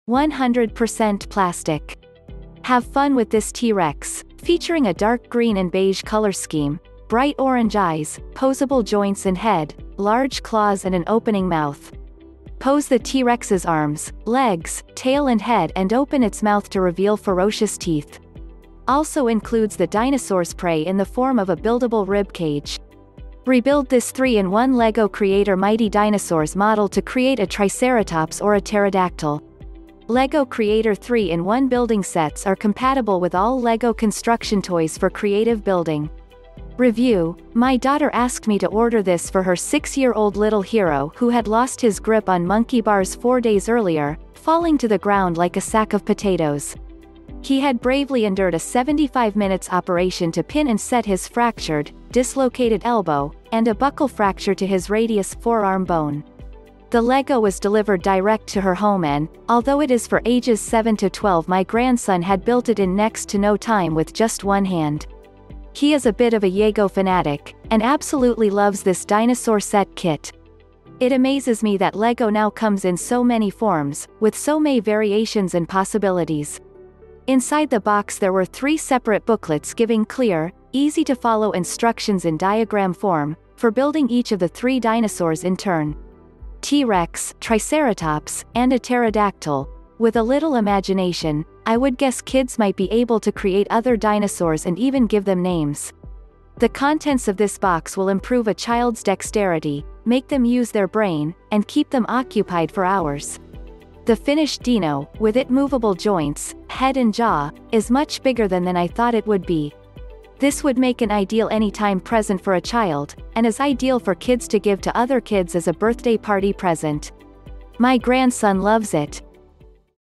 LEGO Creator - Mighty Dinosaurs - 31058
100% Plastic.
Have fun with this T-Rex, featuring a dark-green and beige colour scheme, bright orange eyes, poseable joints and head, large claws and an opening mouth.
Pose the T-Rex's arms, legs, tail and head and open its mouth to reveal ferocious teeth.
Also includes the Dinosaur's prey in the form of a buildable rib cage.
Rebuild this 3-in-1 LEGO creator mighty Dinosaurs model to create a Triceratops or a Pterodactyl.
LEGO creator 3-in-1 building sets are compatible with all LEGO construction toys for creative building. Review:
  My daughter asked me to order this for her 6 year old little hero who had lost his grip on monkey bars 4 days earlier, falling to the ground like a sack of potatoes. He had bravely endured a 75 minutes operation to pin and set his fractured, dislocated elbow, and a buckle fracture to his radius (forearm bone).The Lego was delivered direct to her home and, although it is for ages 7-12 my grandson had built it in next to no time with just one hand. He is a bit of a Llego fanatic, and absolutely loves this dinosaur set/kit. It amazes me that Lego now comes in so many forms, with so may variations and possibilities. Inside the box there were three separate booklets giving clear,easy to follow instructions in diagram form, for building each of the three dinosaurs in turn…. T-Rex, triceratops, and a pterodactyl.With a little imagination, I would guess kids might be able to create other dinosaurs and even give them names. The contents of this box will improve a child’s dexterity, make them use their brain, and keep them occupied for hours. The finished dino, with it movable joints, head and jaw, is much bigger than than I thought it would be. This would make an ideal any time present for a child, and is ideal for kids to give to other kids as a birthday party present. My grandson loves it.